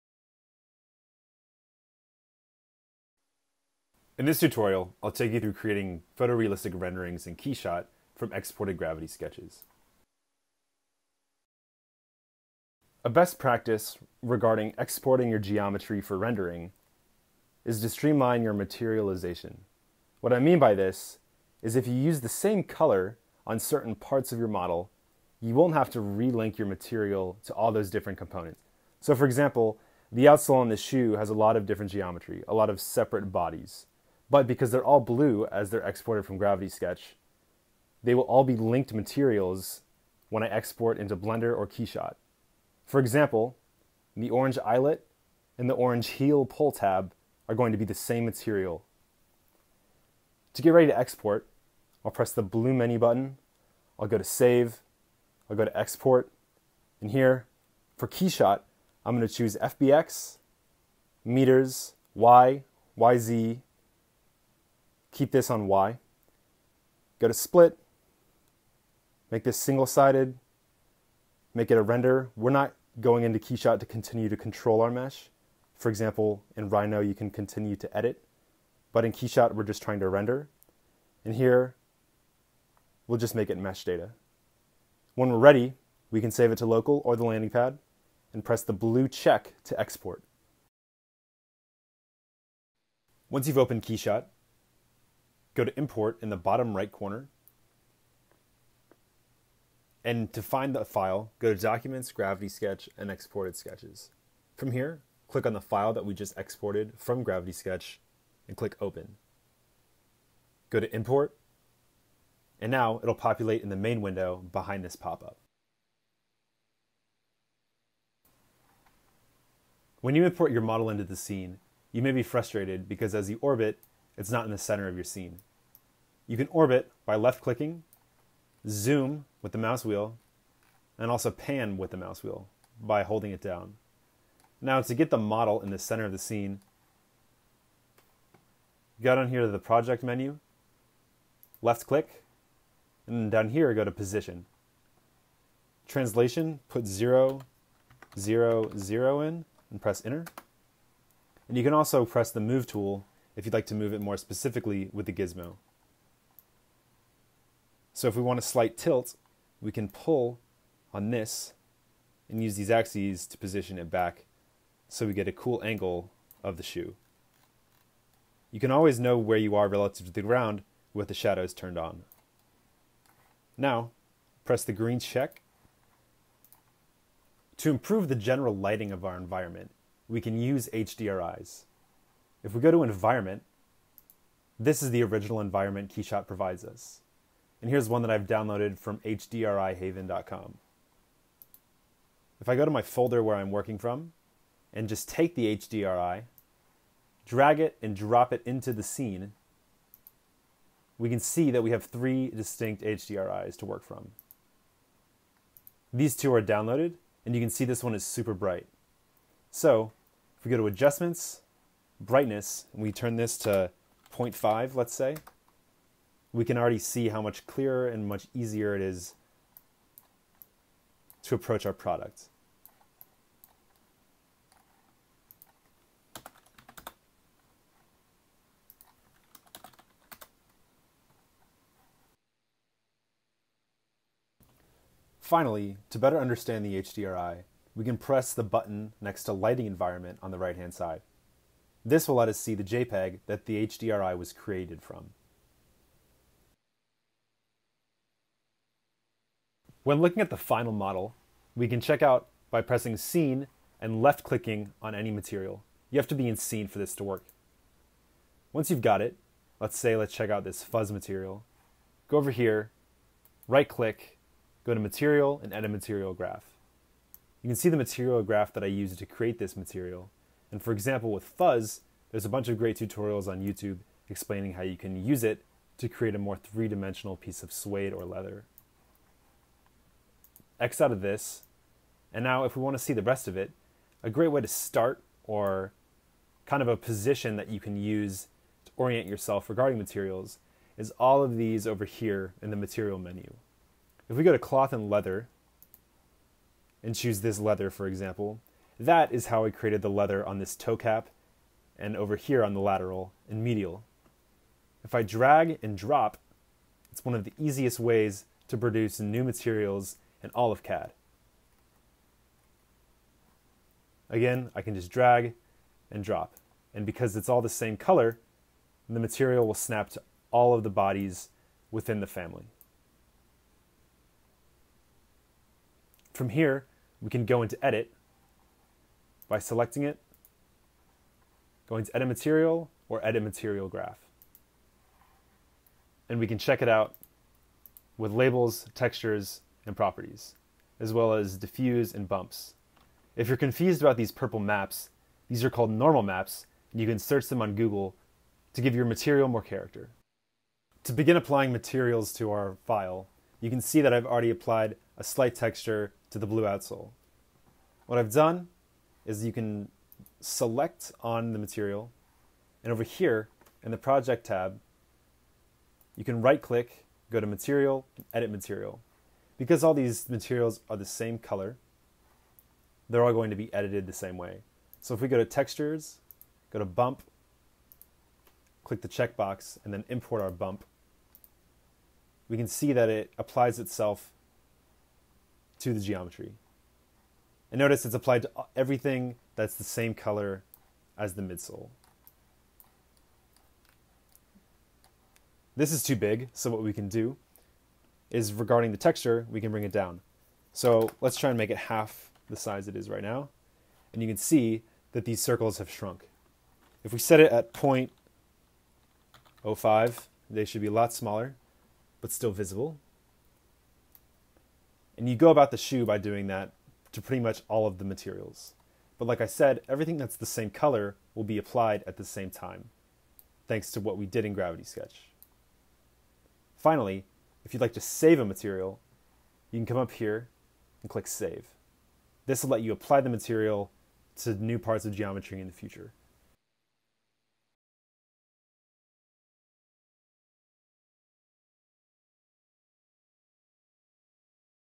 From Gravity Sketch to rendering in KeyShot - Workflow
In this video we show you how to export your models from Gravity Sketch and render them in KeyShot. We cover some of the export settings from GS and then some basic options and tools in KeyShot to begin adding materials and lighting with HDRIs.

This is not meant to be a full tutorial on using KeyShot but rather a way to get started after exporting your models.